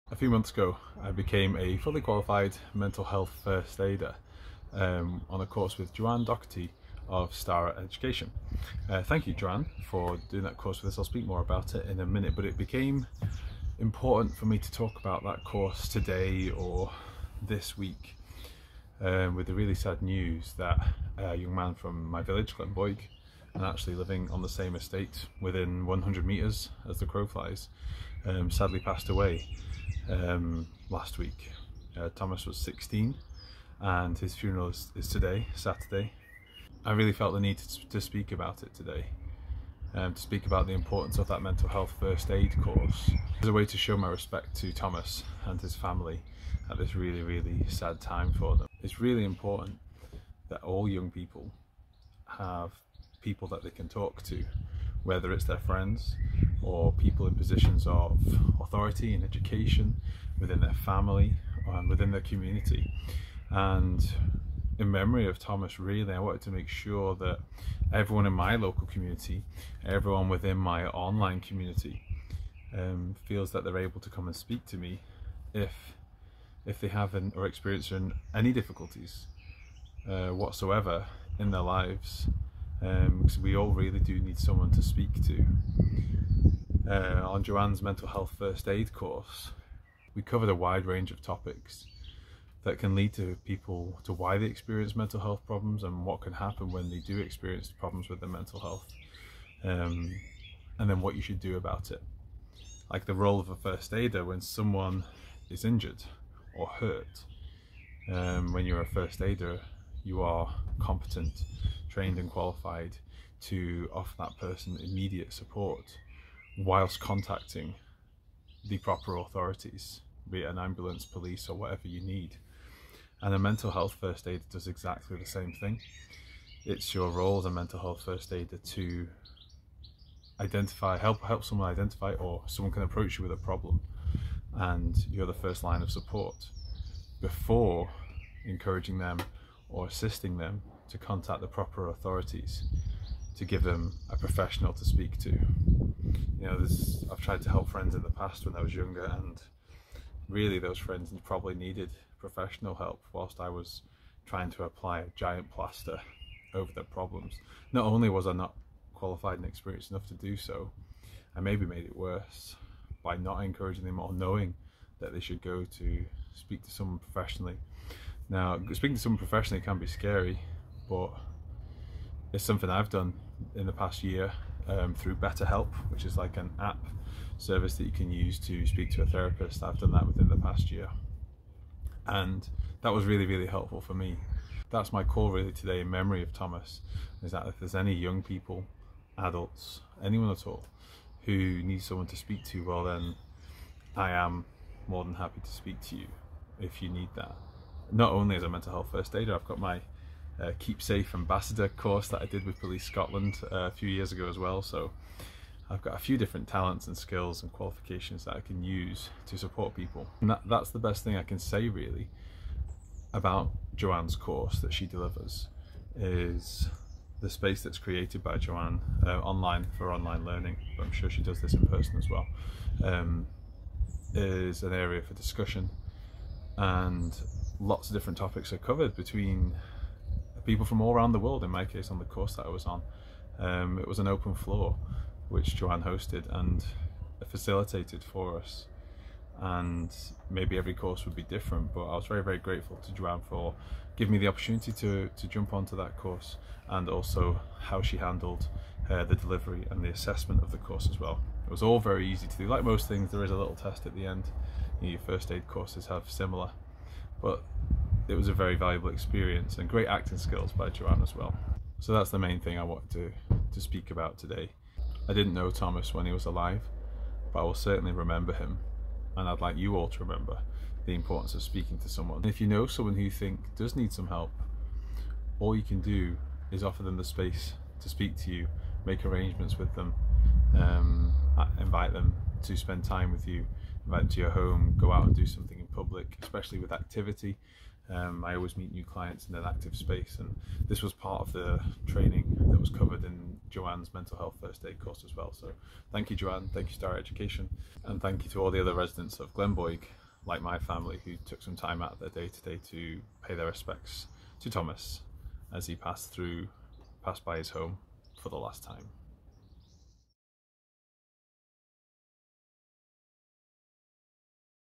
What is Mental Health First Aid and why you should become a mental health first aider with Starra
I'd like to ask for 5 minutes of your time to discuss the importance of mental health first aid, and to recommend a very affordable and highly beneficial mental health first aid course, offered by Joanne Docherty, of Starra Education.

This won't go down in history as the best edited video I've ever submitted to the internet, but I am grateful for anyone who takes the time to watch it. As a Dad of two beautiful children, and related to the circumstances, I found it a little difficult to say what I had to say, so it gets a little choppy in places.

Please know this: as a certified mental health first aider, you can speak to me, in person, or online, if you are finding life difficult, no matter who you are, or what the circumstances are. I am here, competent and qualified to listen and to support you.

If you are interested in becoming a certified mental health first aider yourself, please contact Joanne via DM on Instagram (@starraeducation), or by following visiting her

Inspired by, and in memory of, Thomas Clelland (16, Glenboig).